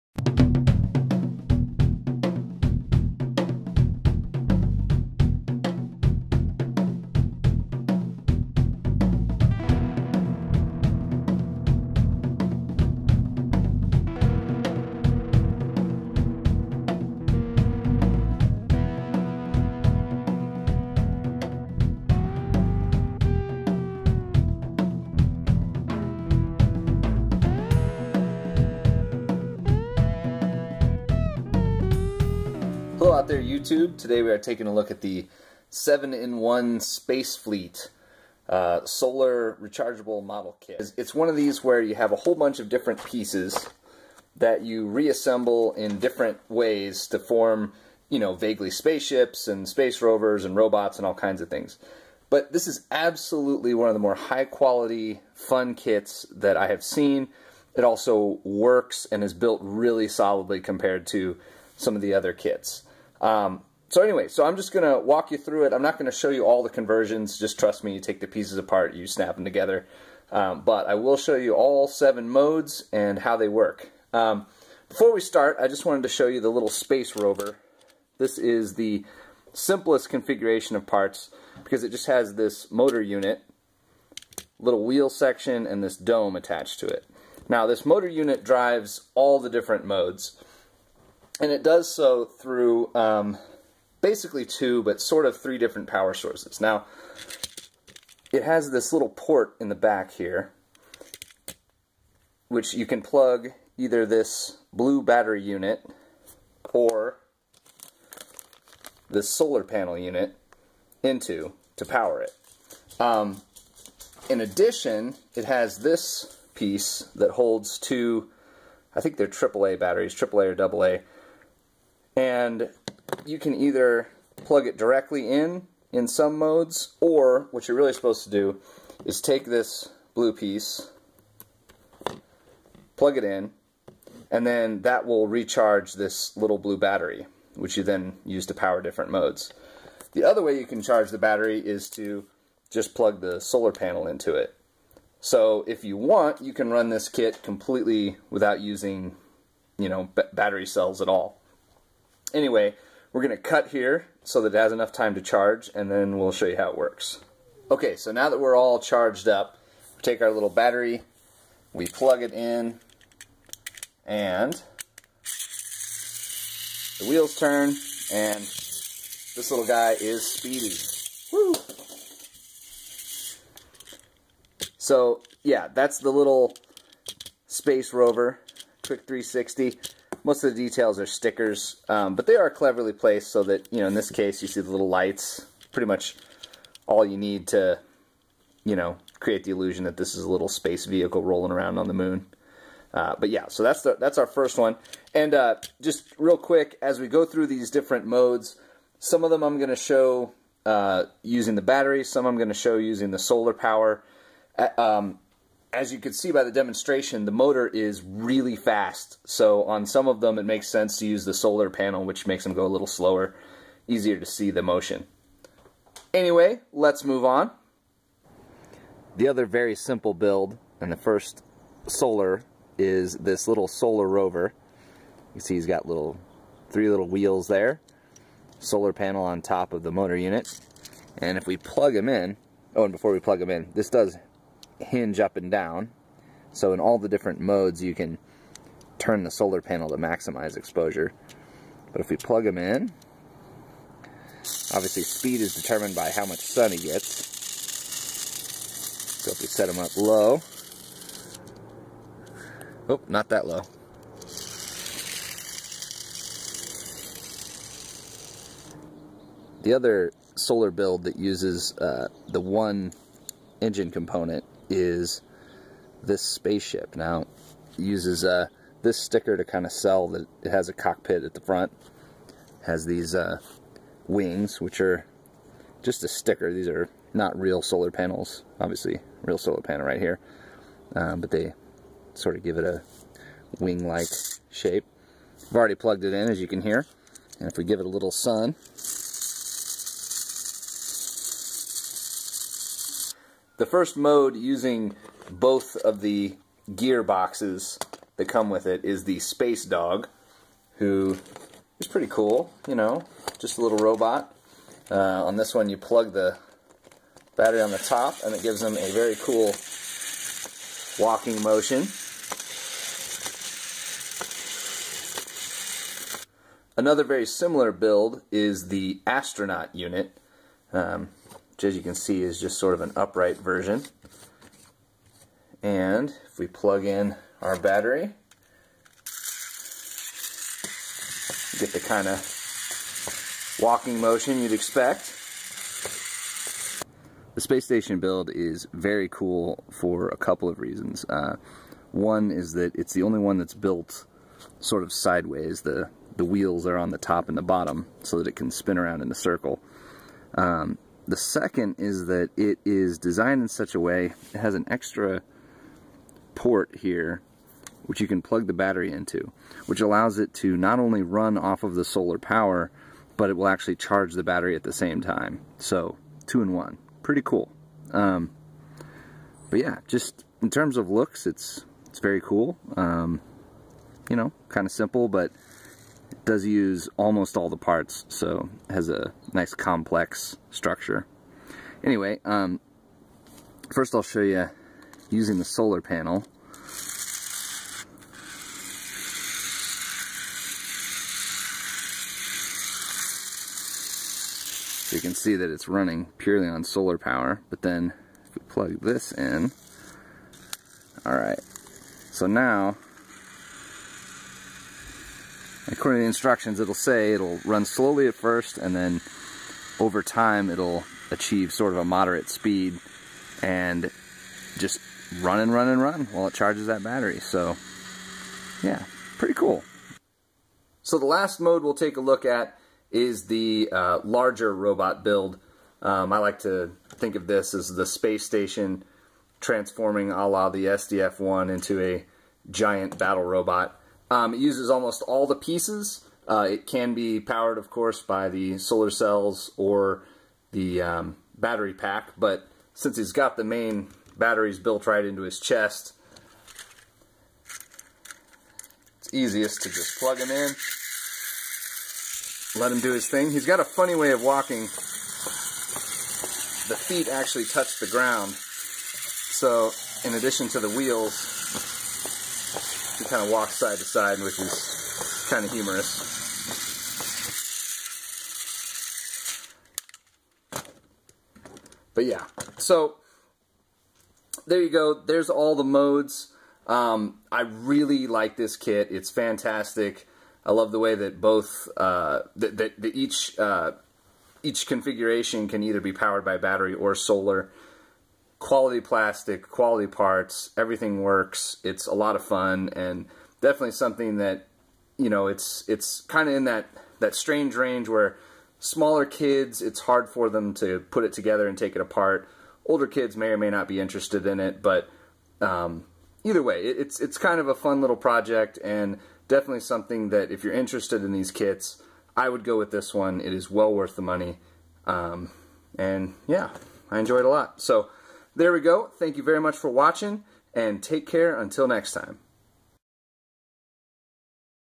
7 in 1 Solar Rechargeable Space Fleet
This is a great high-quality kit which can be reconfigured into 7 distinct modes. It can be powered by either a micro rechargeable battery, a solar panel, or a AAA battery pack. High quality and the best kit of its type I have seen.